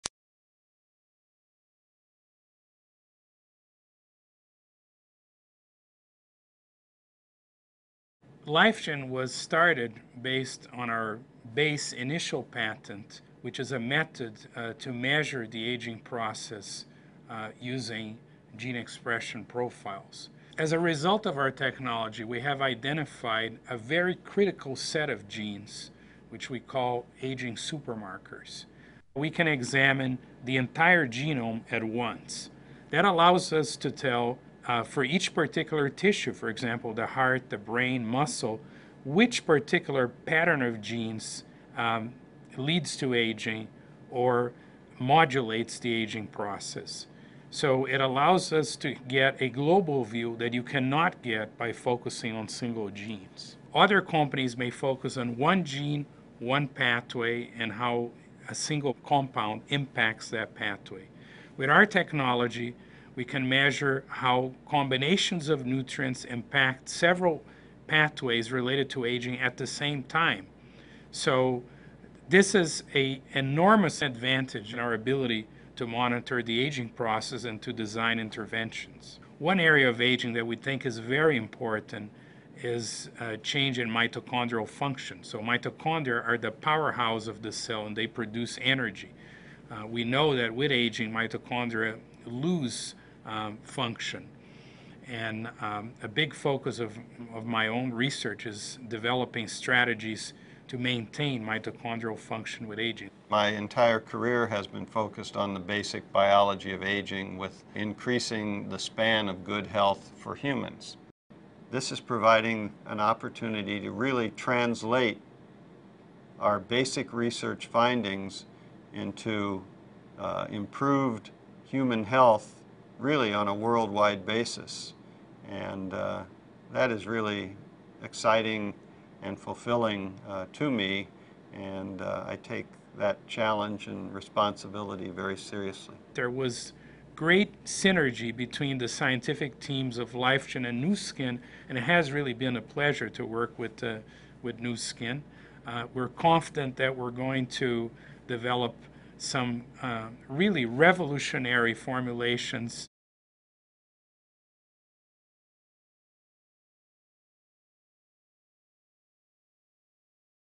The secret is in your genes mp4.mp4
LifeGen scientist explain the significance of gene expression